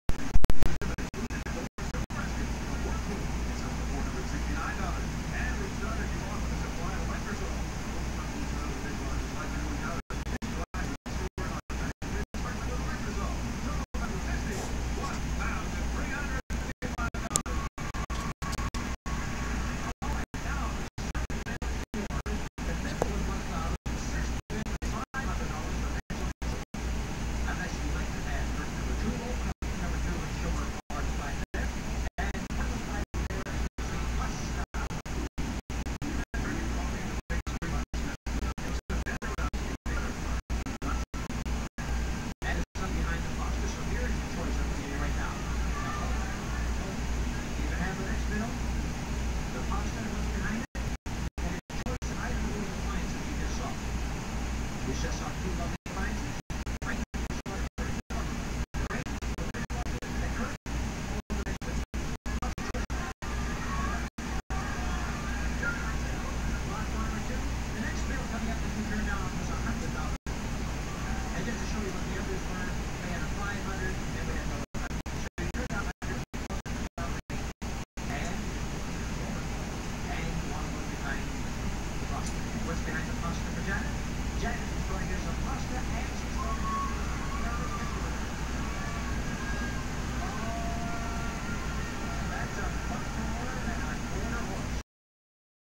My weird dog asleep
Sleeps with her eyes open